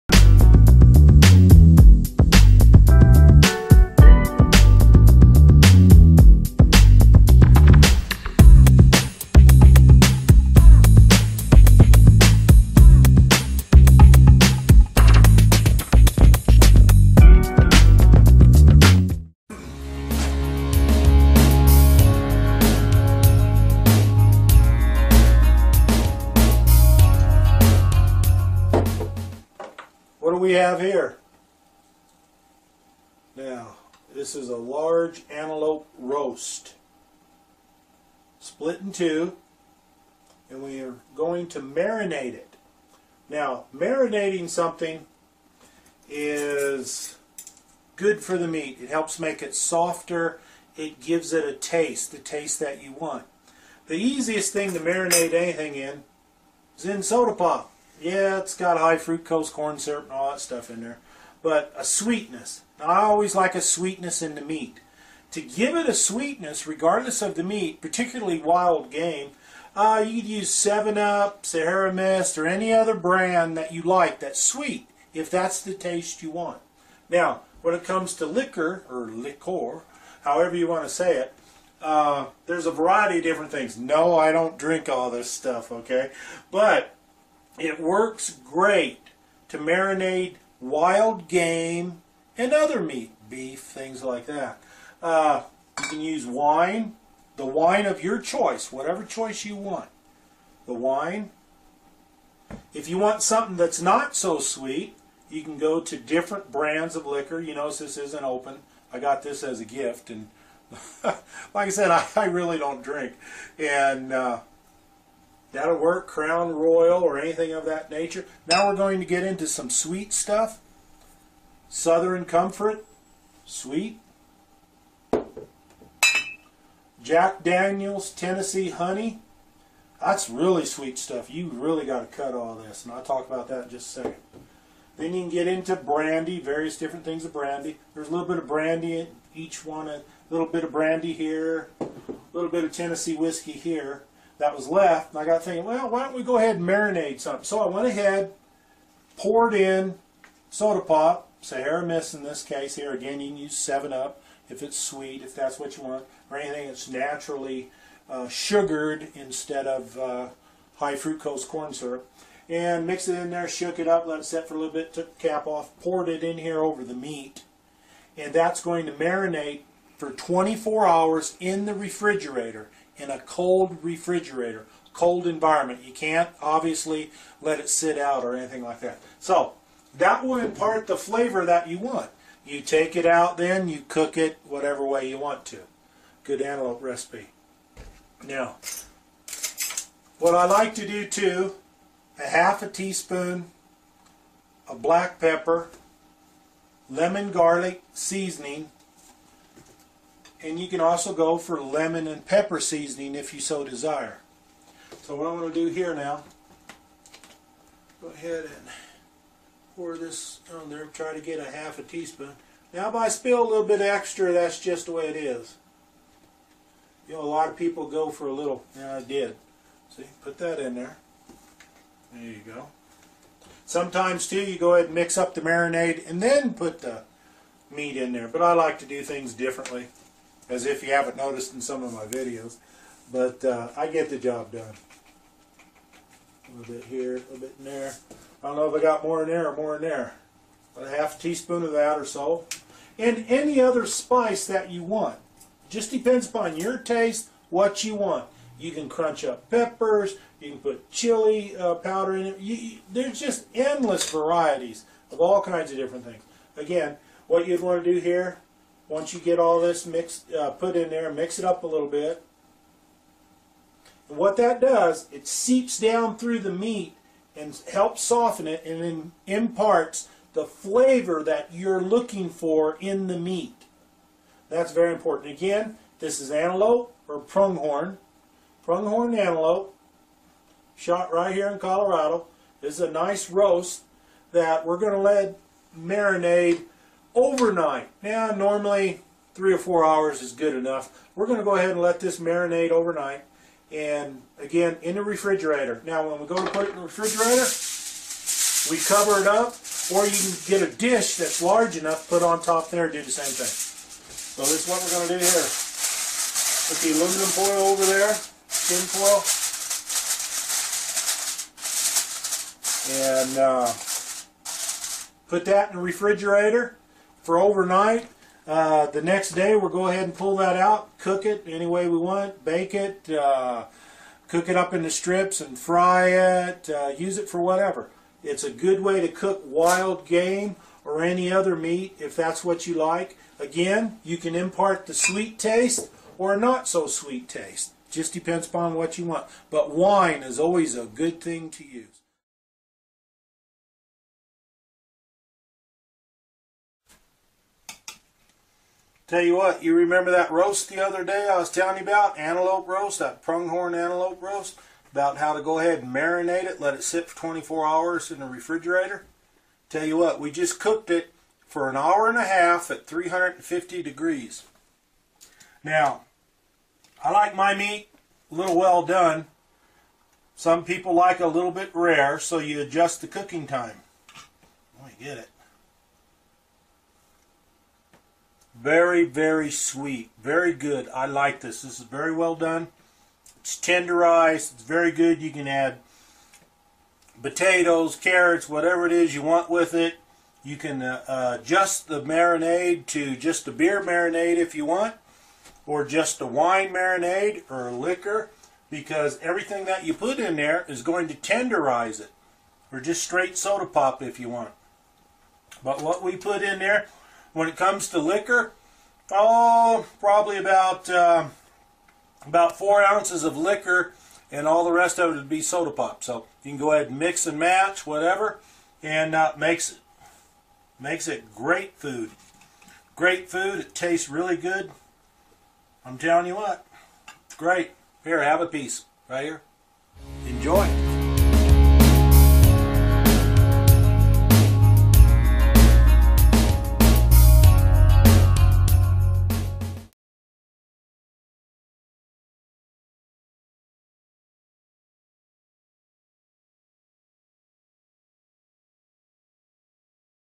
How-to Wild Game Marinade
No more “Wild Game” or gamey taste in your meat.  This video, How-to Wild Game Marinade,  will give you the secrets of making Wild Game Meat taste good!  You are in total control of what you like and don’t like.  It does not matter if the meat is deer, elk, caribou, bear, pronghorn antelope, moose, sheep, goat, hog, or even birds and small game!  If you have a good piece of wild game meat that has been properly cared for by the hunter, you should have no problems with some exquisite tasting meat suitable for the most demanding palate. The ideas and methods put forth in this video work, and are time proven by many years of experience by thousands of people from all cultures and locations.